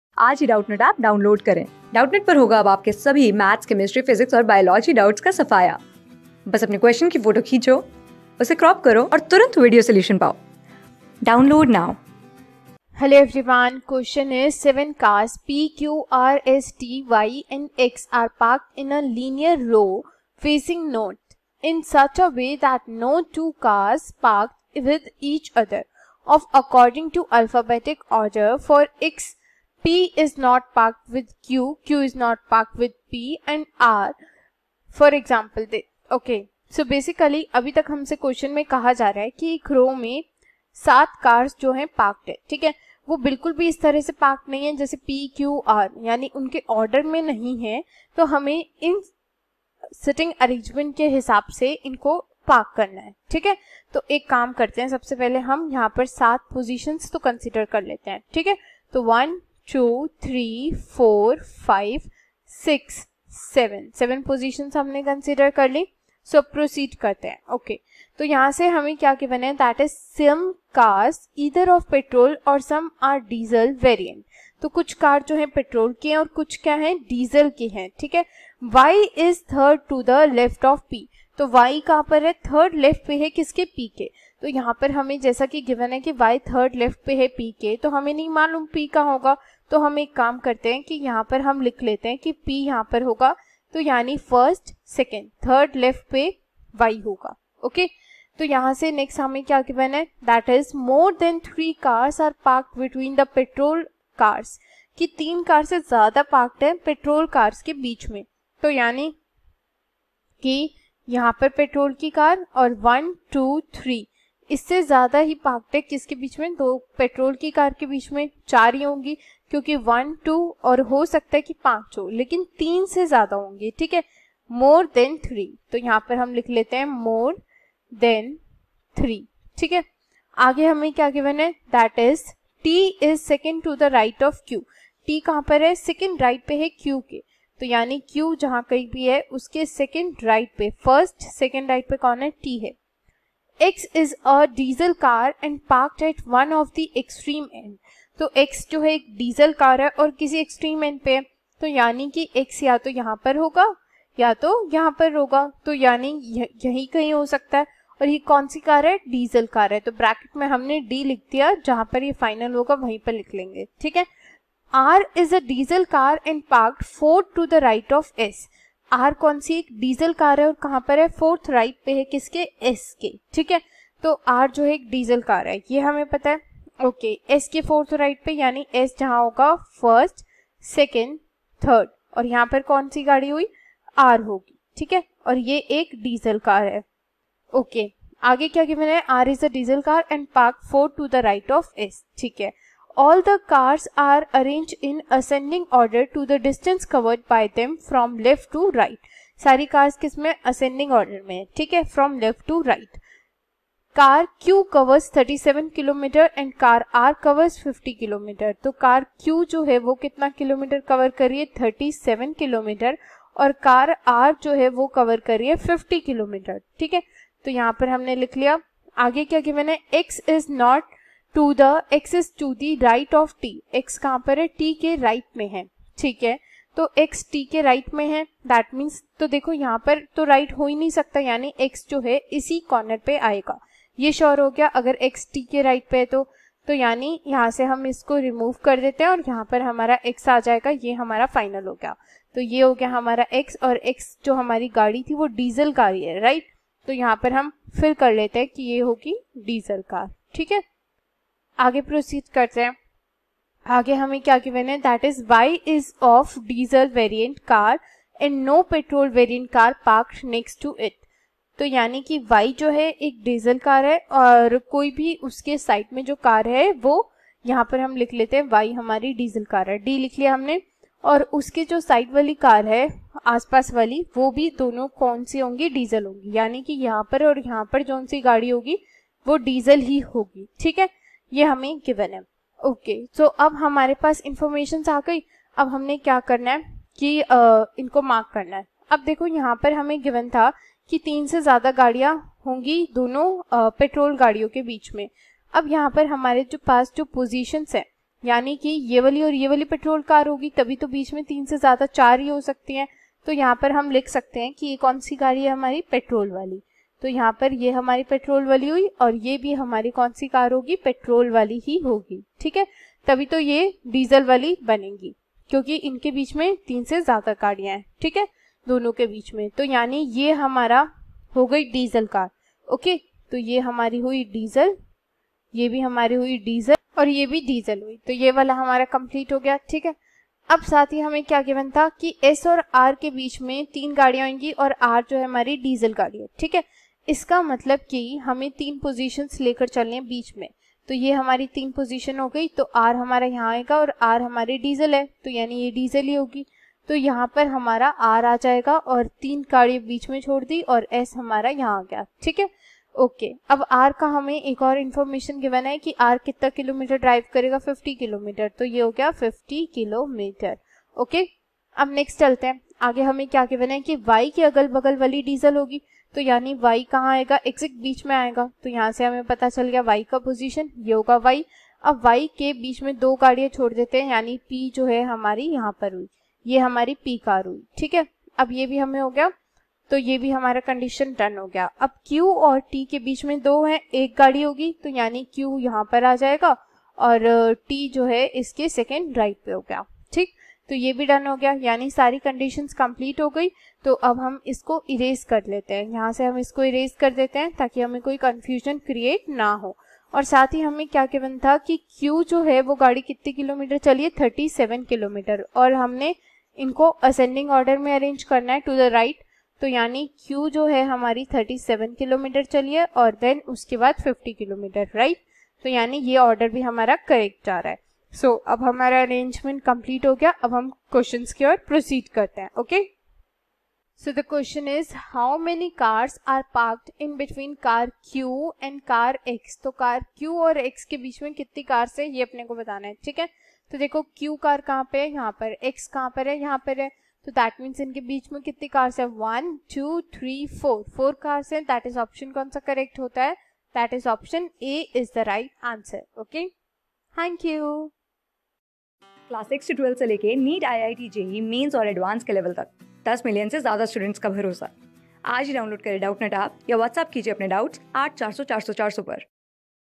Seven cars P, Q, R, S, T, Y, and X are parked in a linear row facing north in such a way that no...
Seven cars P, Q, R, S, T, Y, and X are parked in a linear row facing north in such a way that no two cars parked with each other of according to alphabetical order (for ex-P is not parked with Q, Q is not parked with P and R and so on). Some cars either of Petrol or some are Diesel variant. Y is third to the left of P. More than three cars are parked between the petrol cars. T is second to the right of Q. X is a diesel car and parked at one of the extreme end. R is a diesel car and parked forth to the right of S. All the cars are arranged in ascending order according to the distance covered by them from left to right. Car Q covers 37 km and Car R cover 50 km. X is to the right of T. Y is of Diesel variant car and no petrol variant car parked next to it. How many cars are parked in between car Q and car X ?
Class: 14
Subject: REASONING
Chapter: PROBLEM SOLVING
Board:GOVT

👉 Have Any Query? Ask Us.